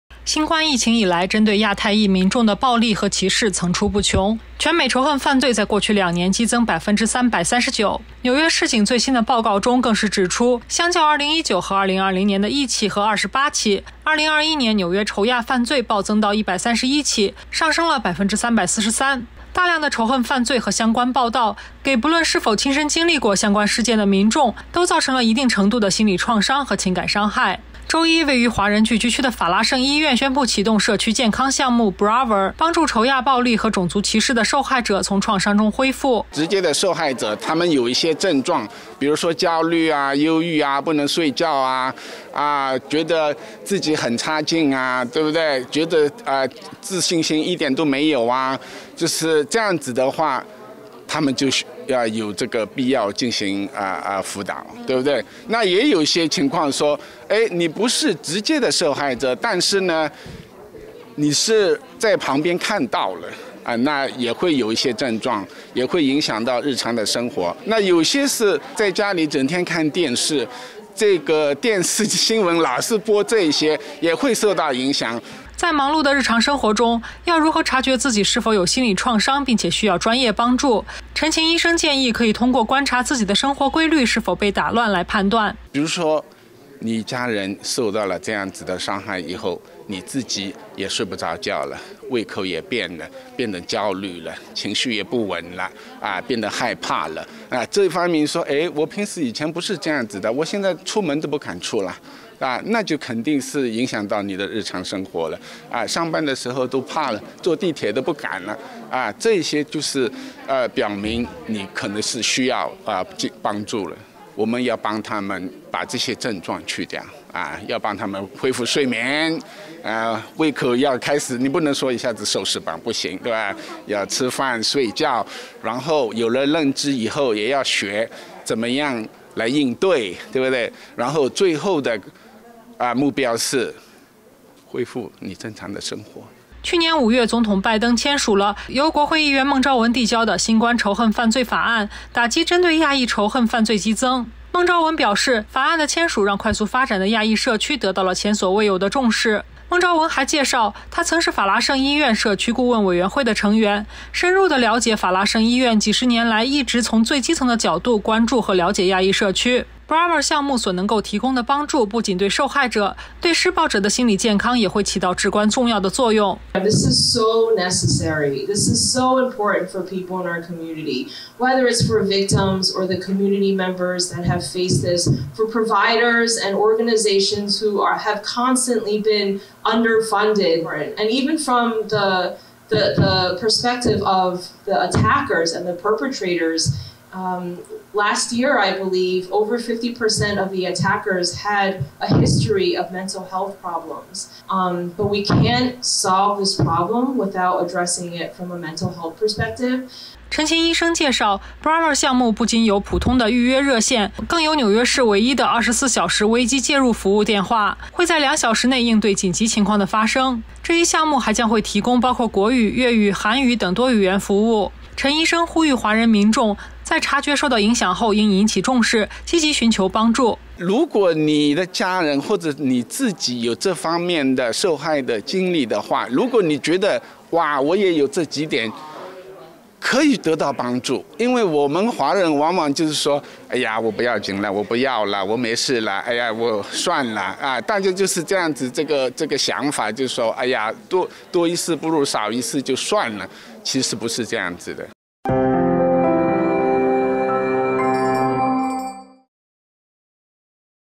纽约法拉盛医院推社区健康项目 助仇亚暴力受害者心理恢复【美国华人圈】
美国中文网记者郭奕纽约报道 新冠疫情以来，针对亚太裔民众的暴力和歧视层出不穷。全美仇恨犯罪在过去两年激增339%。纽约市警最新的报告中更是指出，相较2019和2020年的1起和28起，2021年纽约仇亚犯罪暴增到131起，上升了343%。大量的仇恨犯罪和相关报道，给不论是否亲身经历过相关事件的民众都造成了一定程度的心里创伤和情感伤害。